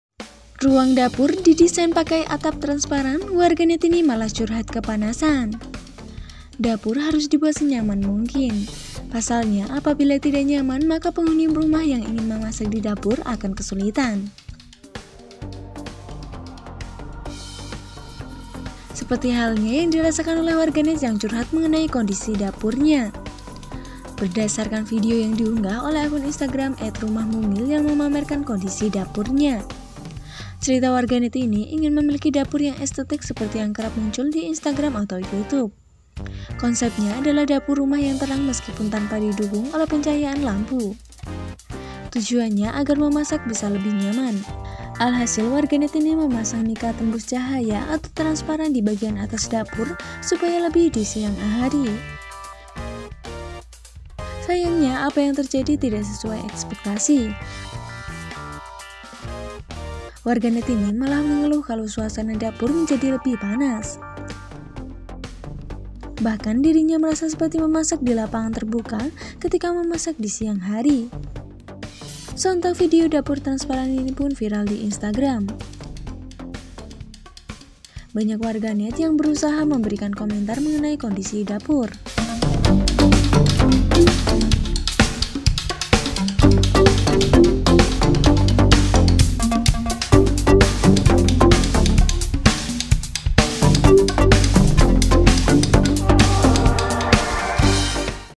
Ruang Dapur Didesain Pakai Atap Transparan, Warganet Ini Malah Curhat Kepanasan
Dapur harus dibuat senyaman mungkin. Pasalnya apabila tidak nyaman, maka penghuni rumah yang ingin memasak di dapur akan kesulitan. Seperti halnya yang dirasakan oleh warganet yang curhat mengenai kondisi dapurnya.

Berdasarkan video yang diunggah oleh akun Instagram @rumahmungil yang memamerkan kondisi dapurnya. Ceritanya warganet ini ingin memiliki dapur yang estetik seperti yang kerap muncul di Instagram atau YouTube.

Vo/Video Editor: Ema/Fatikha Rizky Asteria N